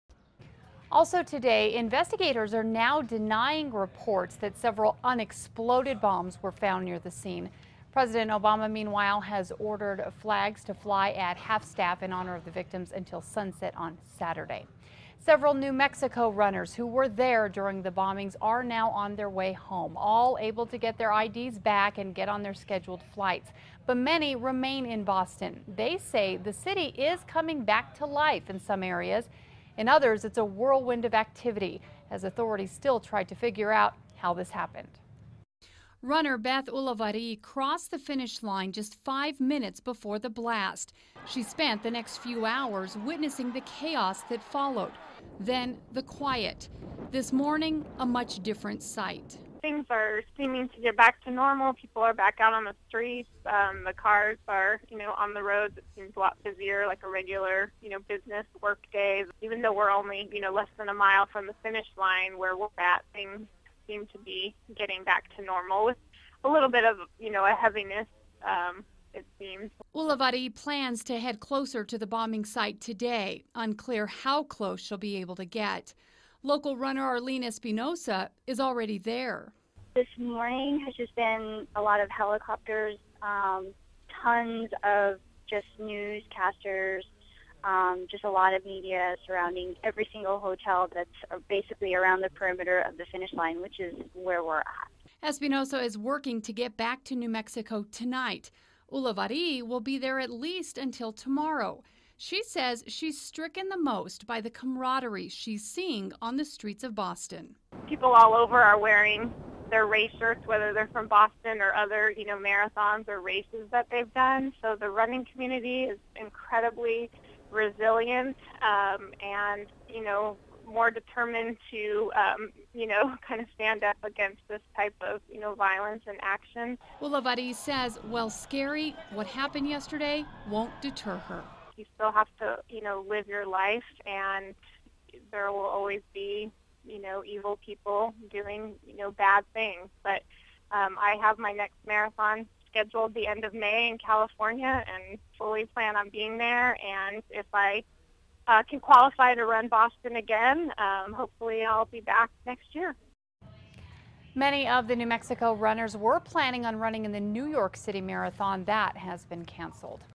N.M. runners remain in Boston
While many New Mexican's who ran in the Boston Marathon made their way home Tuesday, those who remain say life is slowly getting back to normal.